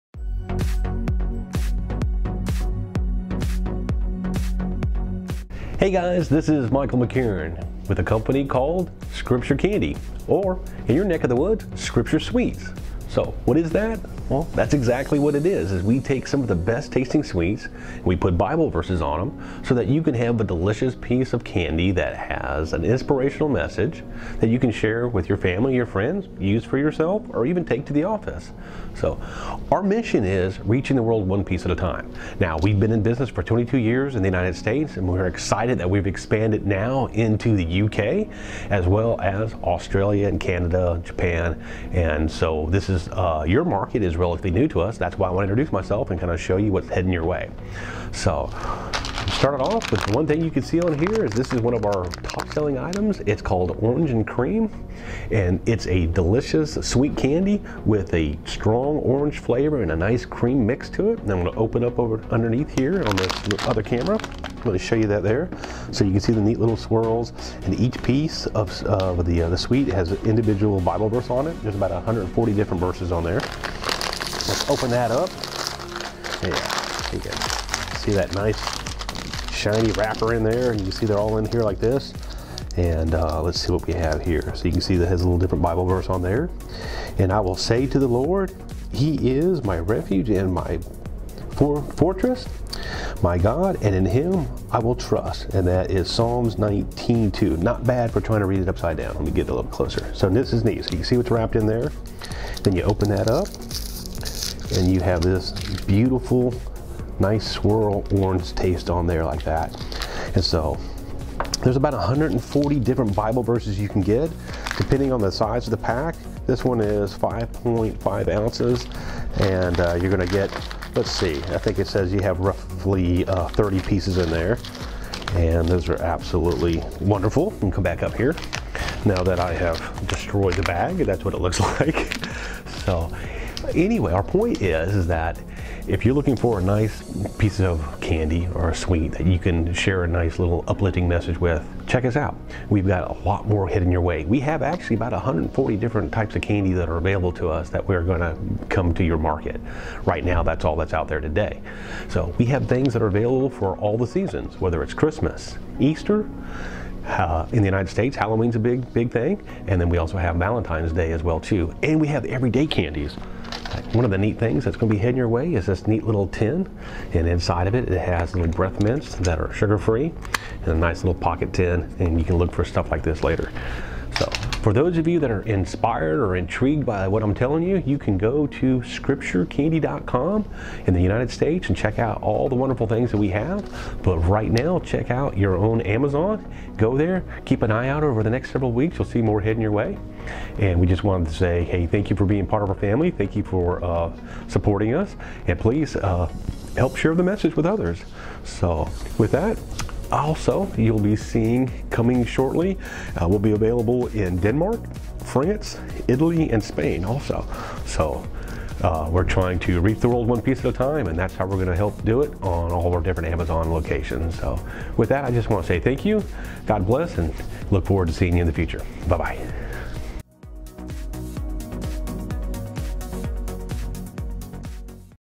Scripture Candy Now In The UK
We here at Scripture Candy are excited to announce our expansion into Amazon United Kingdom. We have several items that will be made available soon but in the meantime check out our Orange & Cream Sweets!